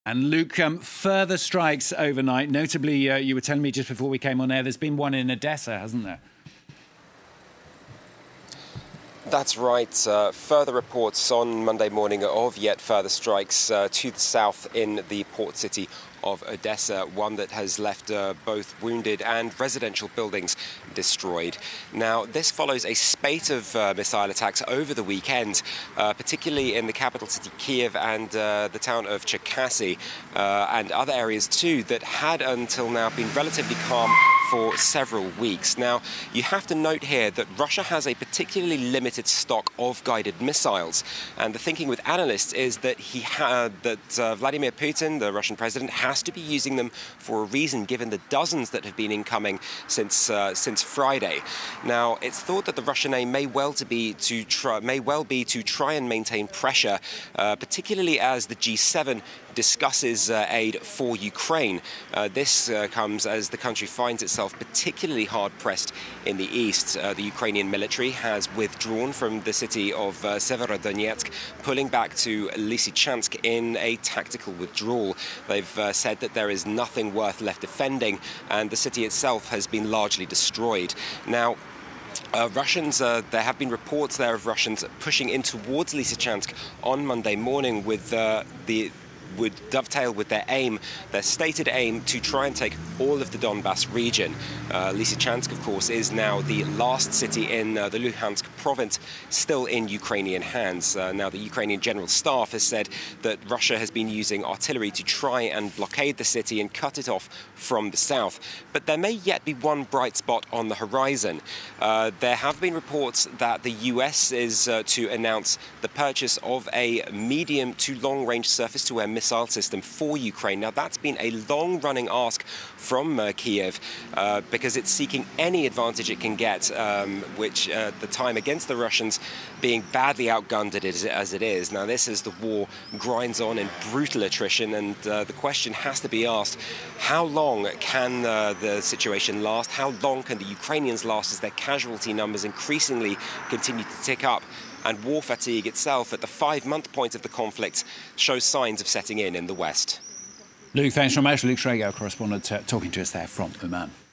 Russian forces concentrate on Lysychansk, the last city still in Ukrainian hands in Luhansk
Russian forces were fighting on Monday to capture Lysychansk, the last major city still held by Ukrainian troops in eastern Luhansk province, after Moscow-backed separatists said they were advancing on multiple fronts. FRANCE 24’s Luke Shrago reports from the Ukrainian city of Uman.